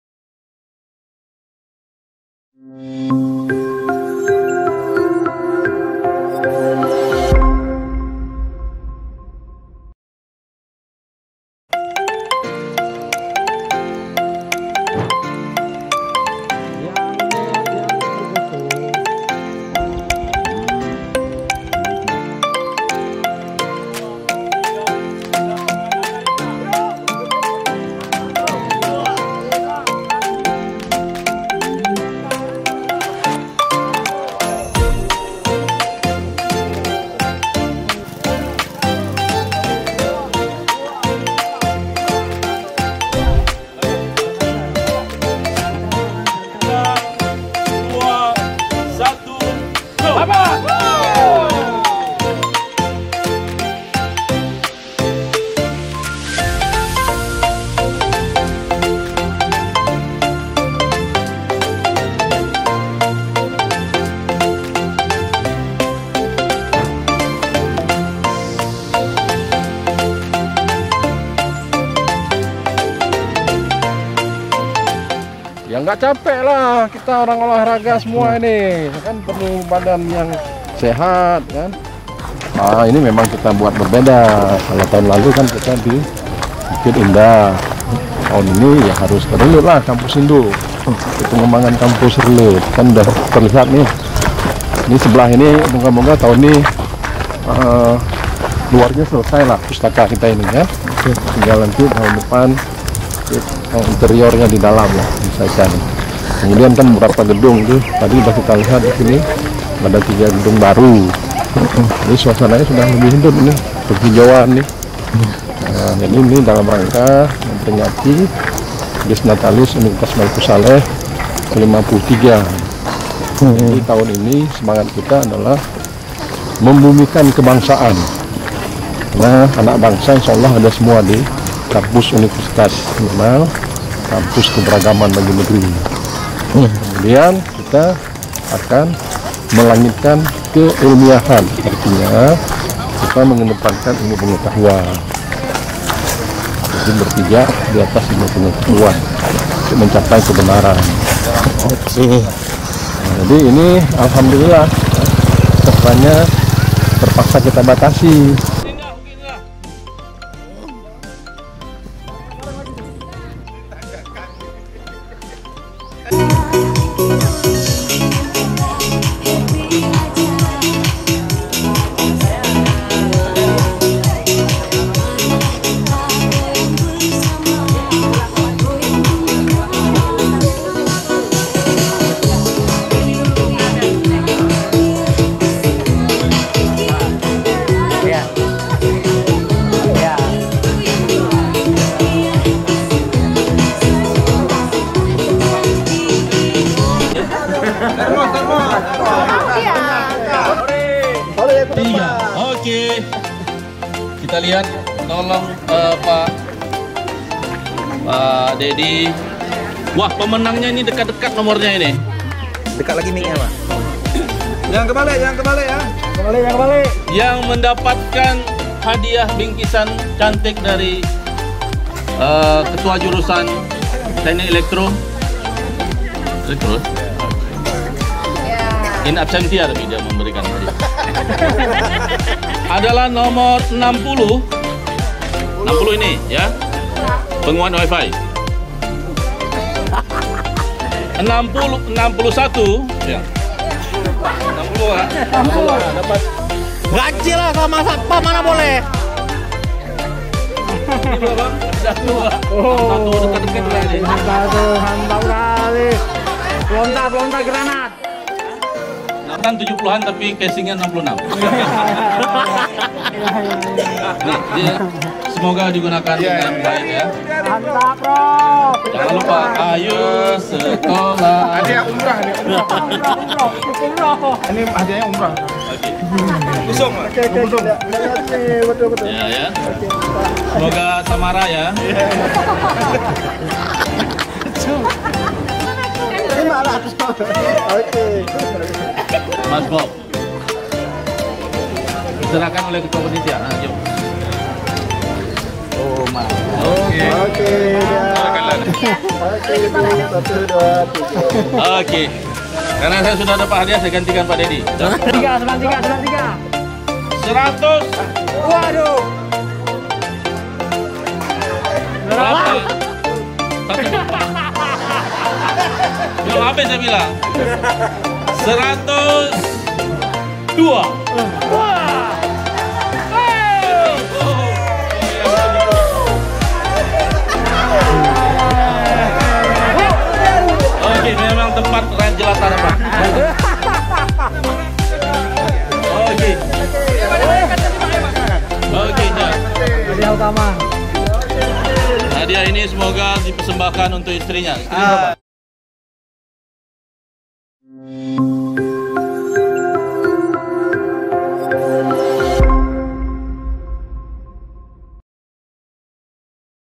Kemeriahan Jalan Sehat Dalam Rangka Dies Natalis Unimal Ke-53
Jalan sehat ini merupakan rangkaian acara Dies Natalis UNimal Ke-53, kegiatan ini diadakan pada hari Minggu, 5 Juni 2022 di Kampus utama Reuleut. kegiatan ini diikuti oleh 70 Peserta diantaranya seluruh Dosen, Tendik dan Pengamanan yang berkerja di Unimal.